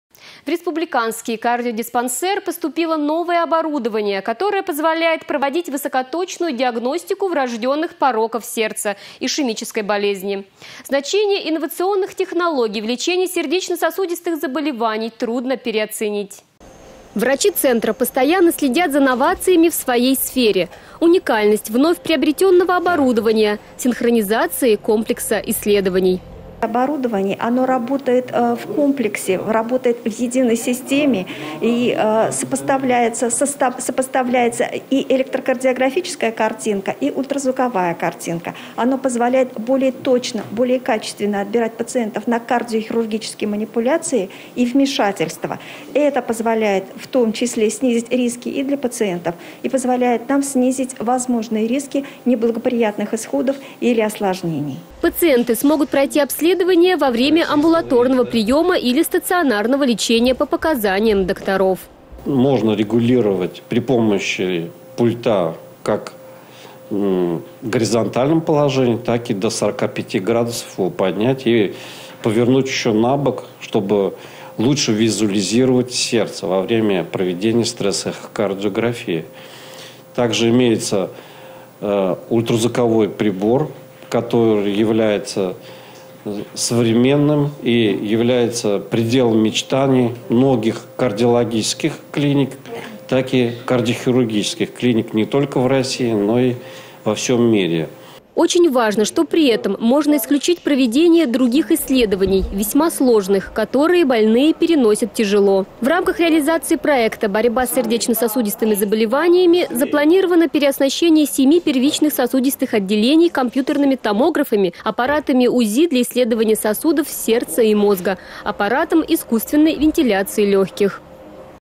В кардиодиспансер поступило новое оборудование
В Республиканский кардиодиспансер поступило новое оборудование, которое позволяет проводить   высокоточную диагностику врожденных пороков сердца, ишемической болезни. Значение инновационных технологий в лечении сердечно-сосудистых заболеваний трудно переоценить.